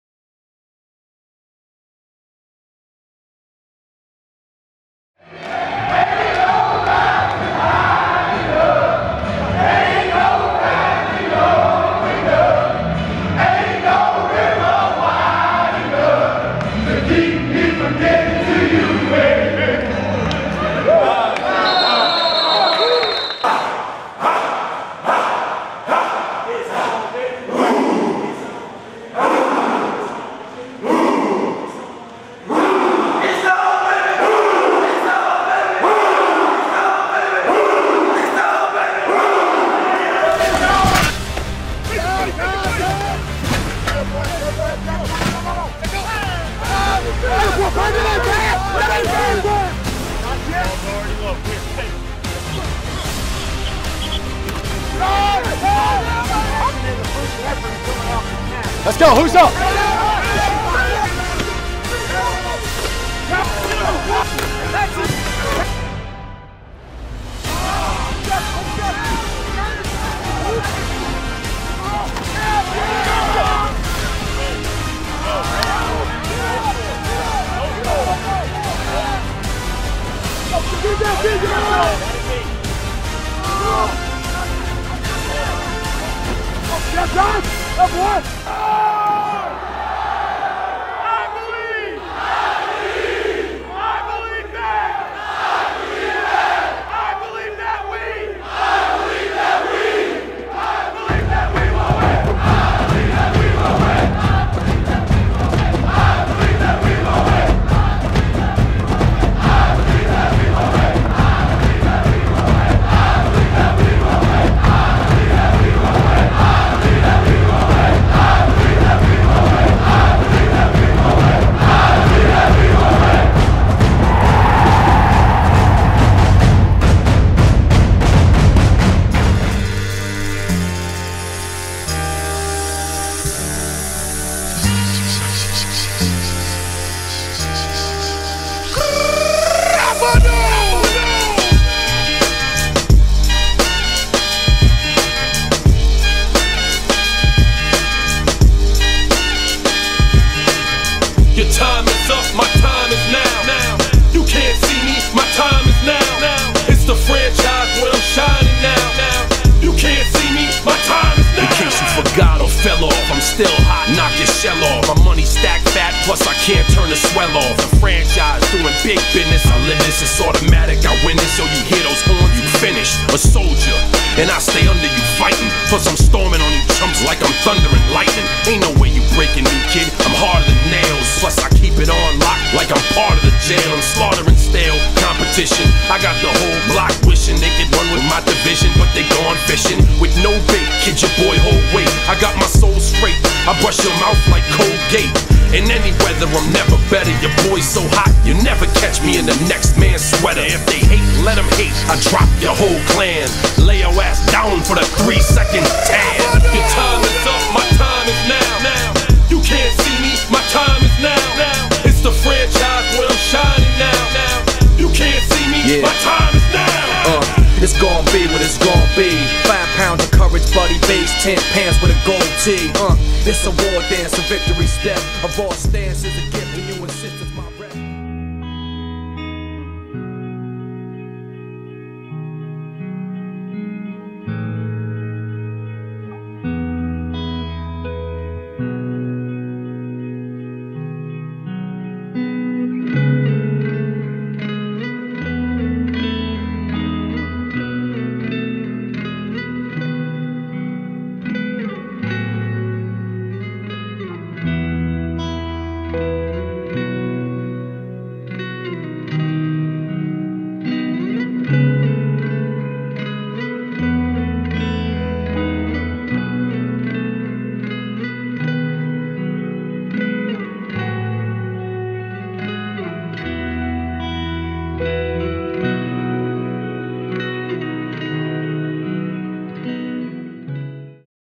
Kennesaw State University Football Hype 2015
Kennesaw State University will MAKE HISTORY in 2015 by taking the Gridiron for the very first time in school history. Lets Go OWLS!!!

Clips are from KSU Owl Vision and Night Owl Productions